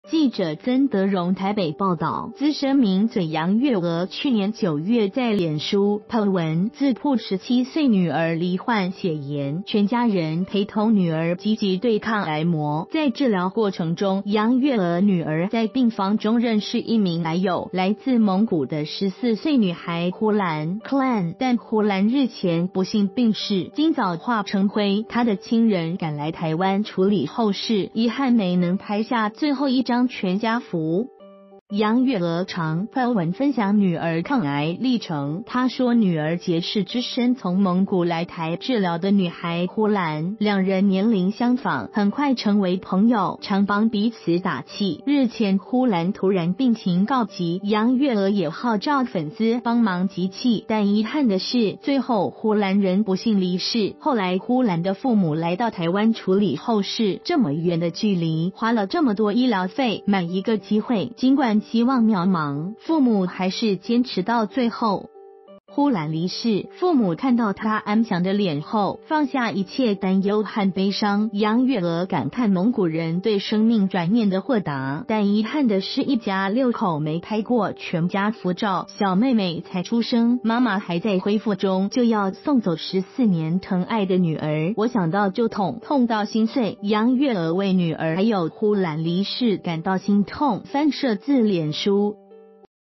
14歲蒙古女孩隻身來台化成灰 名嘴慟哭「心碎」
14歲蒙古女孩隻身來台化成灰 名嘴慟哭「心碎」
〔記者曾德蓉／台北報導〕資深名嘴楊月娥去年9月在臉書po文，自曝17歲女兒罹患血癌，全家人陪同女兒積極對抗癌魔，在治療過程中，楊月娥女兒在病房中認識一名癌友，來自蒙古的14歲女孩「呼蘭（Khulan）」，但呼蘭日前不幸病逝，今早化成灰，她的親人趕來台灣處理後事，遺憾沒能拍下最後一張全家福。

發表評論評論如下。